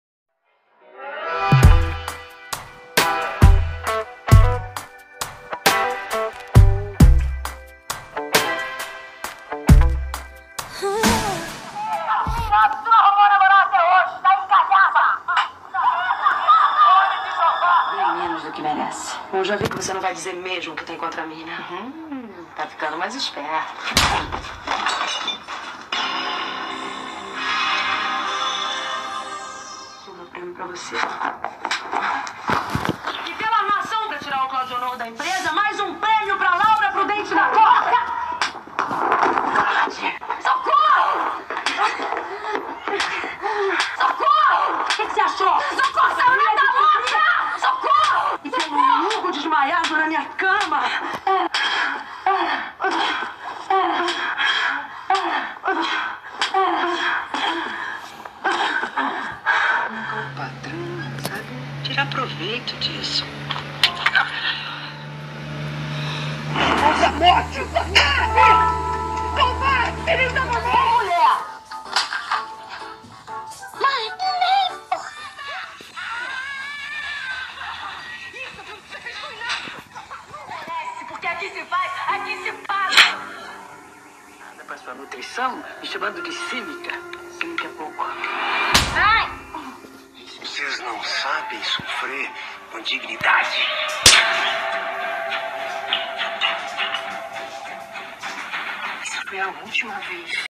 TAPAS COM CLÁUDIA ABREU
Alguns tapas que Cláudia abreu deu e levou nas suas novelas globais

Videos de segunda a sexta